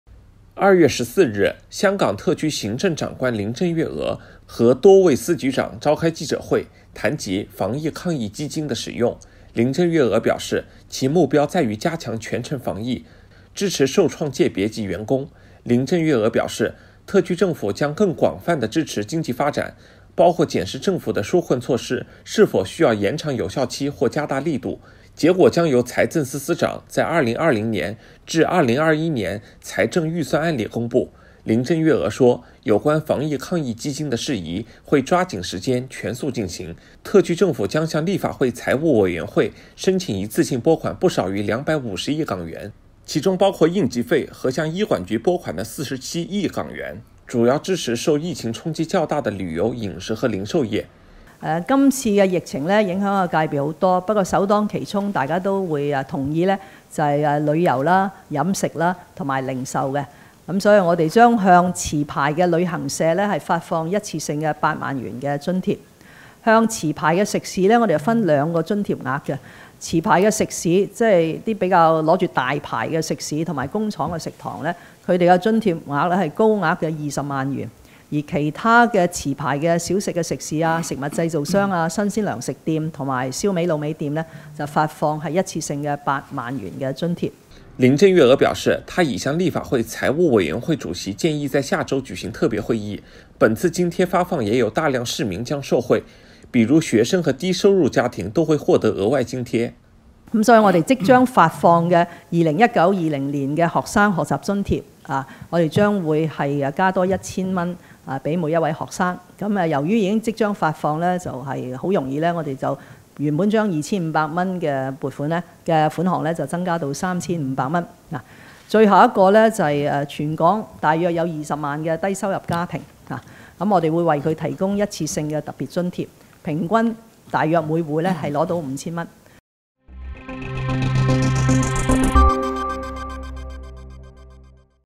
林鄭月娥：防疫抗疫基金目標在支持受創界別及員工
2月14日，香港特區行政長官林鄭月娥和多位司局長召開記者會，她表示，防疫抗疫基金的目標在於加強全城防疫、支持受創界別及員工。林鄭月娥表示，特區政府將更廣泛地支援經濟發展，包括檢視政府的紓困措施是否需要延長有效期或加大力度，將於2020年至2021年財政預算案由財政司司長公佈。林鄭月娥說，有關防疫抗疫基金的事宜會抓緊時間、全速進行。特區政府將向立法會財務委員會申請一次性撥款不少於250億港元，其中包括應急費和向醫管局撥款的47億港元，主要支援受疫情衝擊較大的旅遊、飲食和零售業。